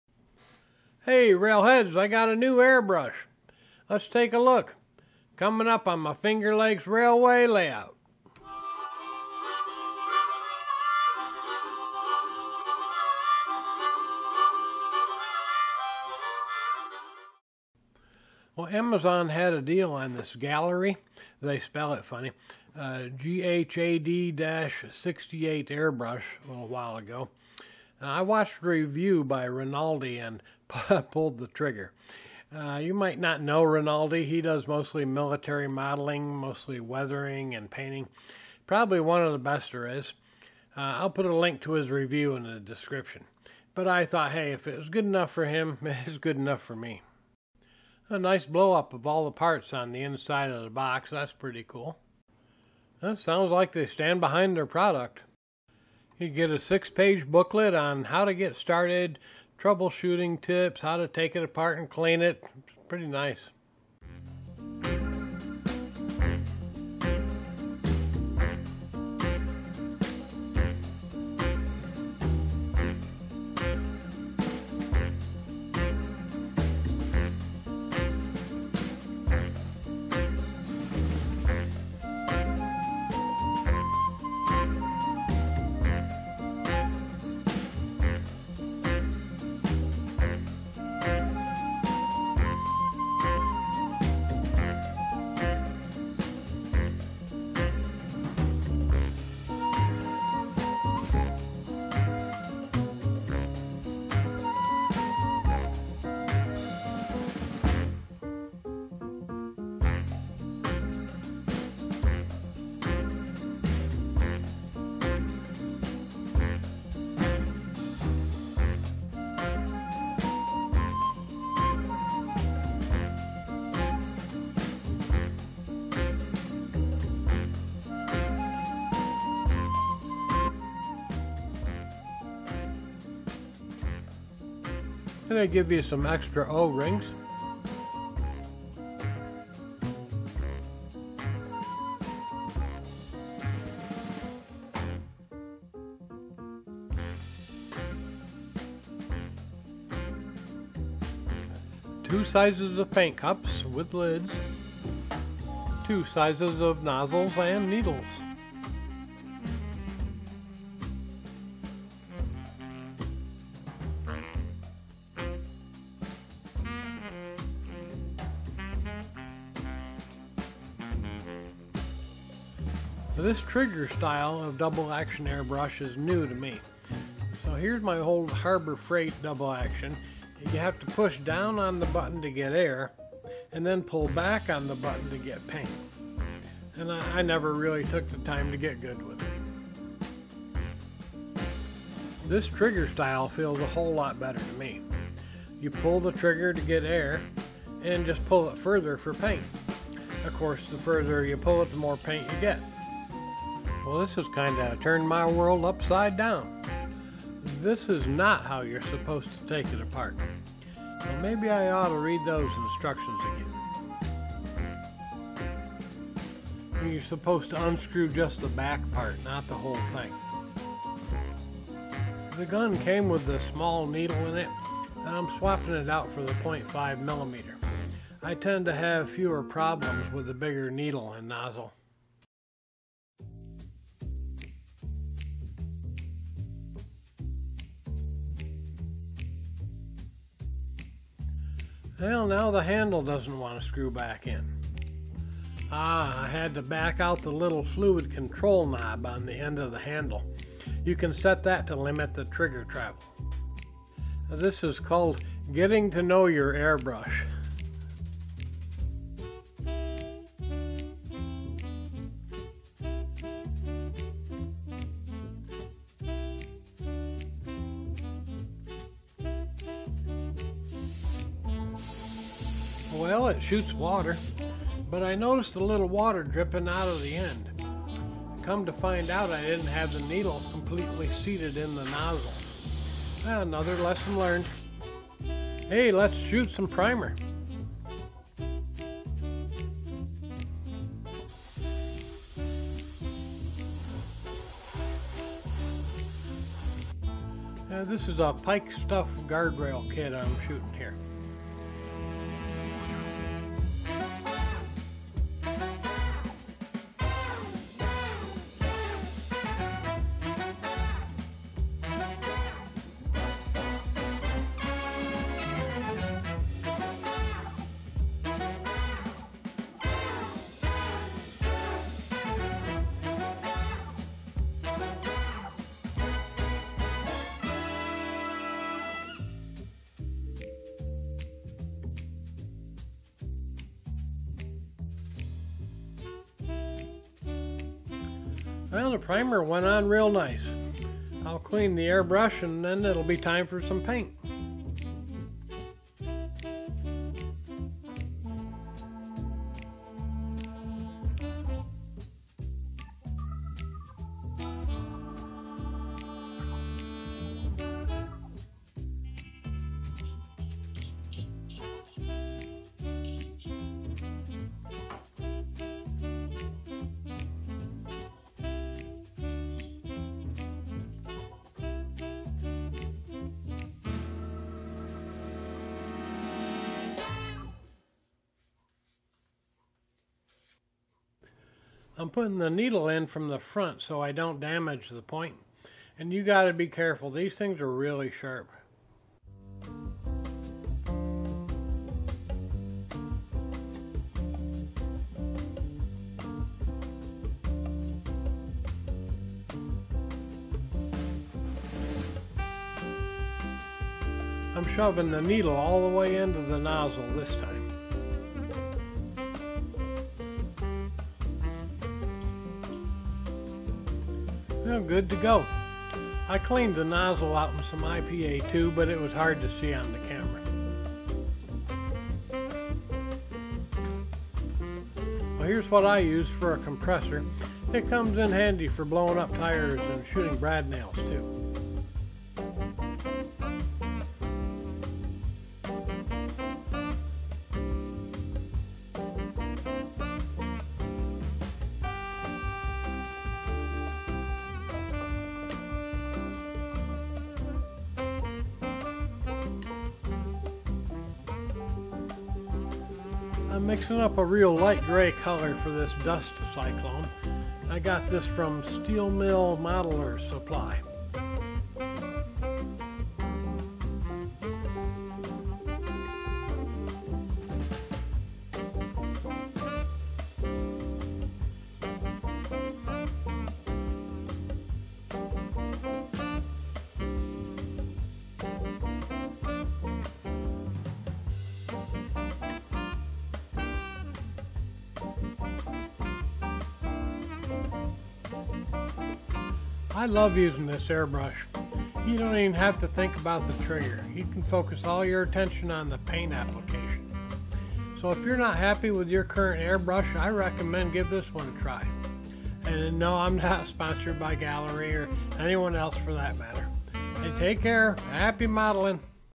Gaahleri Airbrush Review
Finally got a good airbrush that is easy and fun to use!

🔷 Gaahleri GHAD-68 Airbrush on Amazon
🔷 Steel Mill Modelers Supply [My take: Excellent quality industrial 3d print. Took a while to get it, but worth the wait.]

👍🔔Follow My Finger Lakes Railway Layout on social media -

💖Special thanks to Adriel Mourey for the harp music.